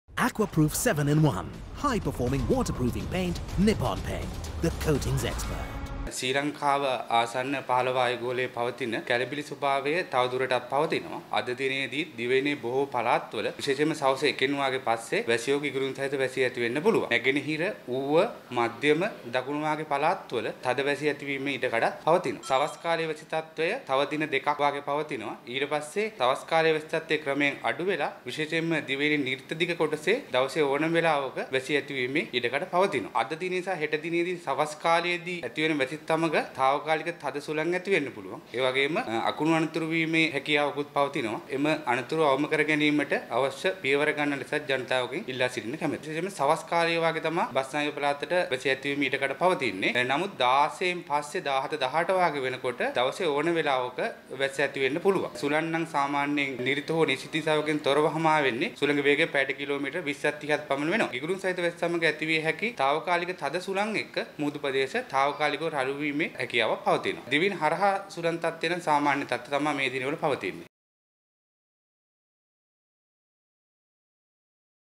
Isolated showers to continue ,Public urged to take precautions from lightning
Isolated showers will continue in parts of the country today and tomorrow, especially in the evening.

The Department of Meteorology also urged the public to take precautions from lightning.